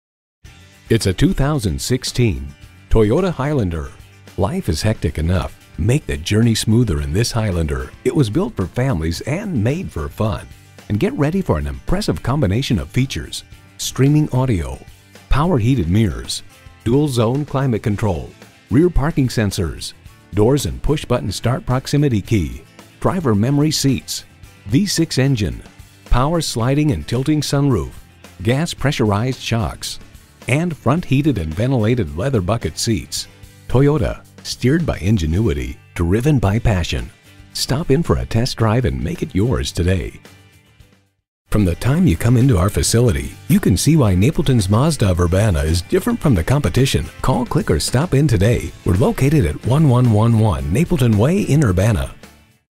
Used 2016 Toyota Highlander Urbana IL Champaign, IL
Year: 2016
Make: Toyota
Model: Highlander
Trim: Limited
Engine: 3.5L V6 DOHC Dual VVT-i 24V
Transmission: 6-Speed Automatic Electronic with Overdrive
Stock #: PT49753
VIN: 5TDDKRFH3GS249753
Priced below KBB Fair Purchase Price! This vehicle is very clean and just serviced!! Fresh Oil change, New Cabin and Air Filters!! Rigorous State and Napleton's Inspection!!

Address:
1111 O'Brien Dr